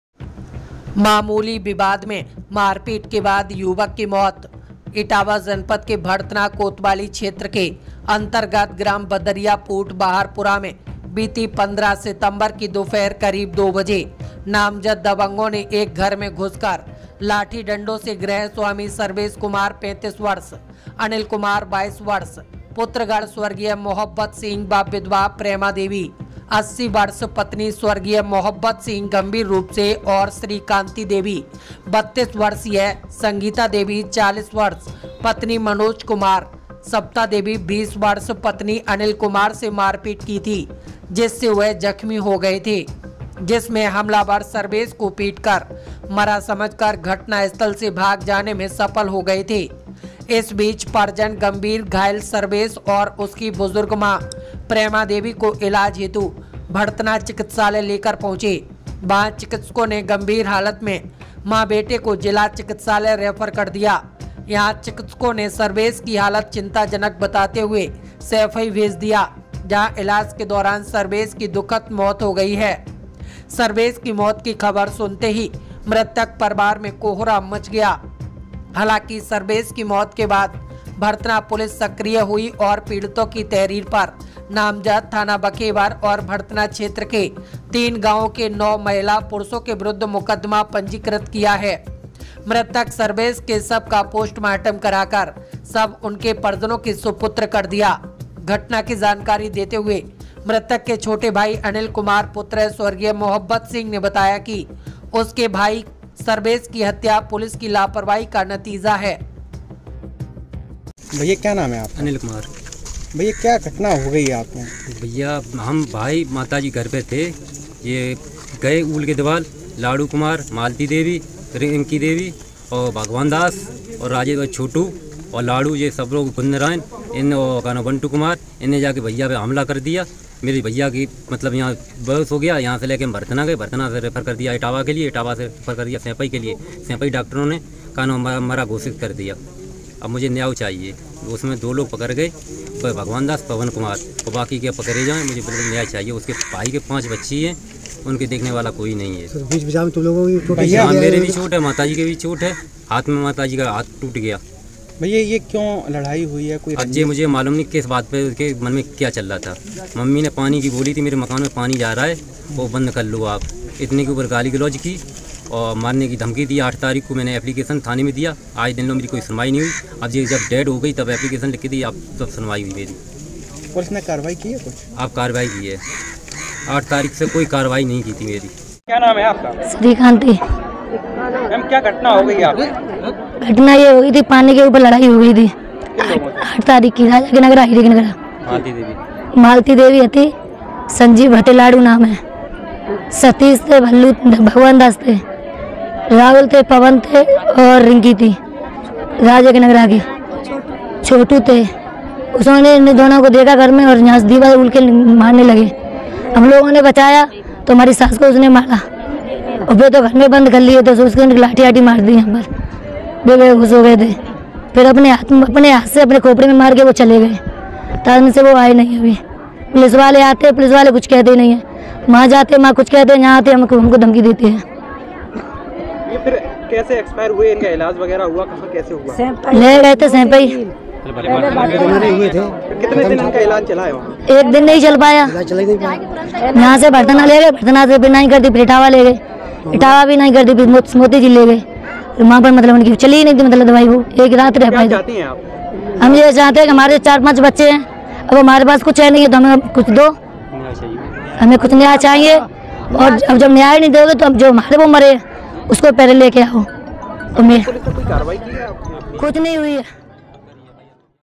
मामूली विवाद में पीट-पीट कर युवक की *त्** - Police 🚓 पर कार्यवाही न करने का आरोप - BNT Etawah
MSS न्यूज़

रिपोर्ट - विष्णु राठौर, पूर्वी यूपी प्रभारी

इटावा यूपी

मामूली विवाद में पीट-पीट कर युवक की हत्**  - पुलिस पर कार्यवाही न करने का आरोप

इटावा में भरथना कोतवाली क्षेत्र के अंतर्गत ग्राम बदरिया पूठ बाहरपुरा में बीती 15 सितम्बर की दोपहर करीब दो बजे नामजद दबंगों ने एक घर में घुसकर लाठी,डंडों से गृहस्वामी सर्वेश कुमार 35 वर्ष , अनिल कुमार 22 वर्ष , पुत्रगण स्व० मोहब्बत सिंह व विधवा प्रेमा देवी 80 वर्ष पत्नी स्व० मोहब्बत सिंह गम्भीर रूप से और श्रीकांति देवी 32, संगीता देवी 40 पत्नी मनोज कुमार,सविता देवी 20 पत्नी अनिल कुमार से मारपीट की थी, जिससे वह जख्मी हो गए थे ।

 जिसमें हमलावर सर्बेश को पीटकर मरा समझकर घटना स्थल से भाग जाने में सफल हो गये थे ।

इस बीच परिजन गम्भीर घायल सर्वेश औऱ उसकी बुजुर्ग मां प्रेमा देवी को इलाज हेतु भरथना चिकित्सालय लेकर पहुँचे वहां चिकित्सकों ने गम्भीर घायल में मां-बेटे को जिला चिकित्सालय रैफर कर दिया, यहां चिकित्सकों ने सर्वेश की हालत चिंताजनक बताते हुए सैंफई भेज दिया, जहां इलाज के दौरान सर्वेश की दुःखद मौत हो गई। सर्वेश की मौत की खबर सुनते ही मृतक परिवार में कोहराम मच गया।

हालांकि सर्वेश की मौत के बाद भरथना पुलिस सक्रिय हुई और पीड़ितों की तहरीर पर नामजद थाना बकेबर और भरथना क्षेत्र के तीन गांवों के 9 महिला पुरुषों के विरुद्ध मुकदमा पंजीकृत किया है । मृतक सर्वेश के शव का पोस्टमार्टम कराकर शव उनके परिजनों के सुपुर्द कर दिया।

घटना की जानकारी देते हुए मृतक के छोटे भाई अनिल कुमार पुत्र स्व०मोहब्बत सिंह ने बताया कि उसके उसके भाई सर्वेश की हत्या पुलिस की लापरवाही का नतीजा है ।

Join & Support MSS NEWS 24X7 News Channel -